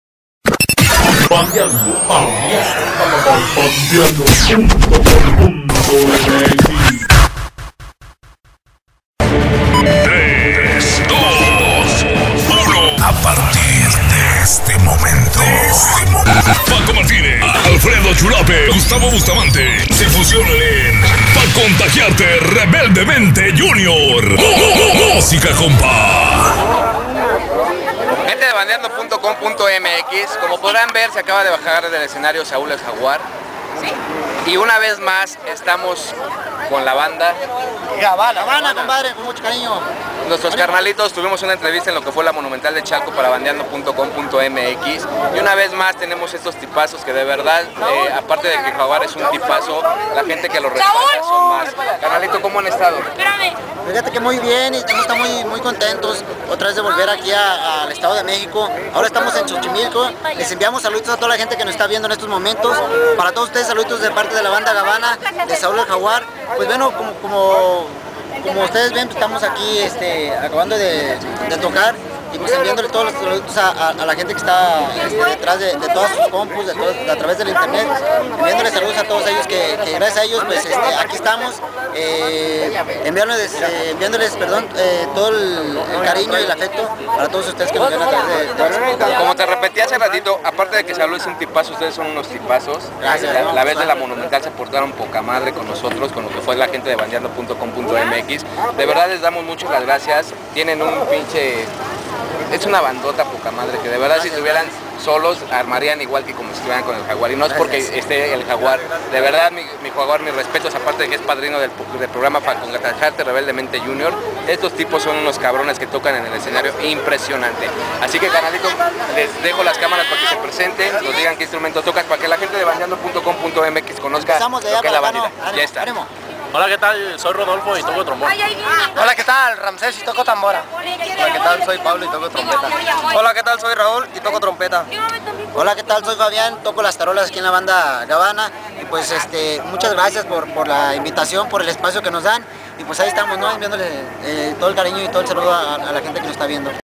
BANDA GABANA DE SAUL EL JAGUAR ENTREVISTA PARA BANDEANDO.FM
BANDEANDO.COM.MX PRESENTE CON BANDA GABANA EN EL RODOE XOCHIMILCO